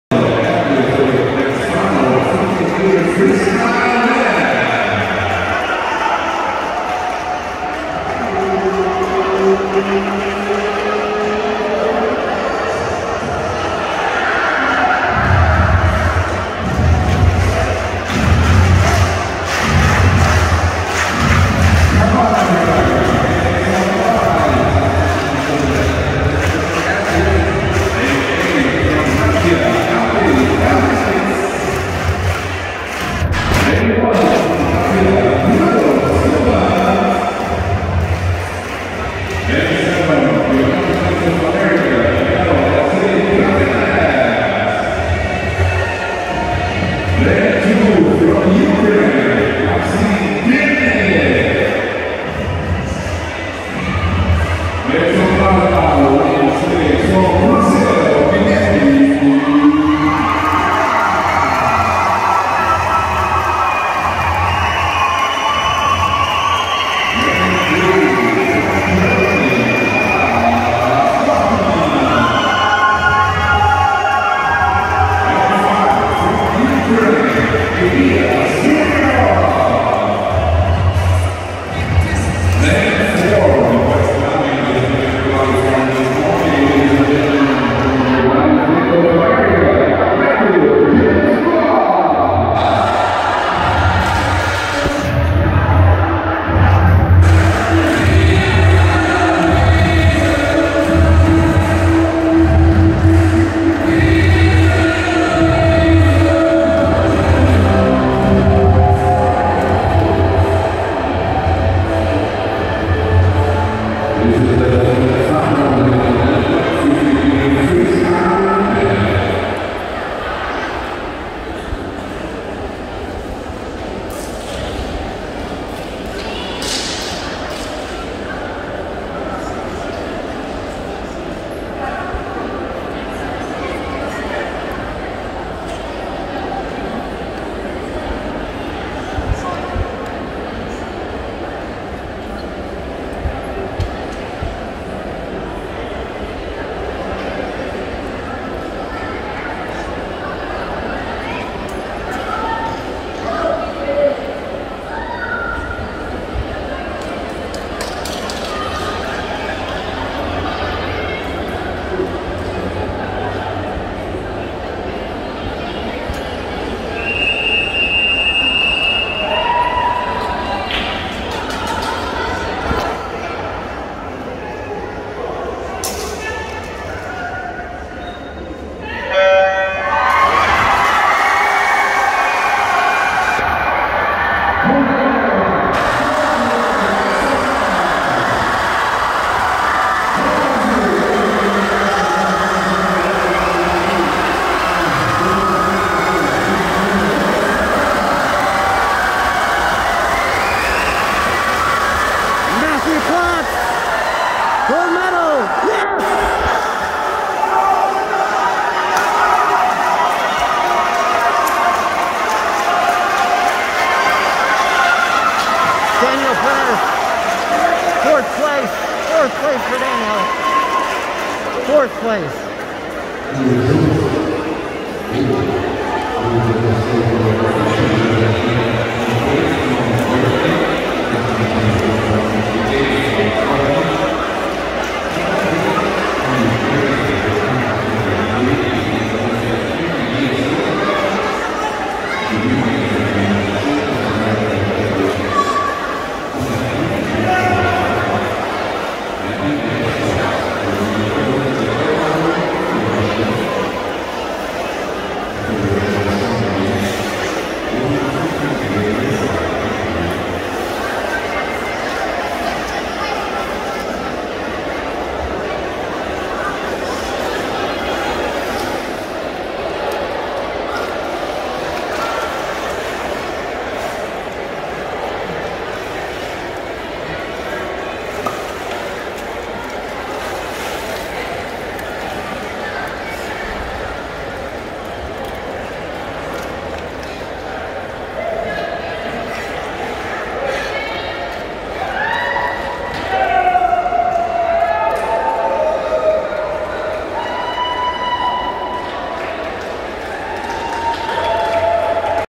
24th Deaflympics Men's 50M Freestyle Finals. Matthew Klotz from the USA wins gold.
Men's 50M Freestyle finals at the 24th Deaflympics. Matthew Klotz from the USA places first and wins gold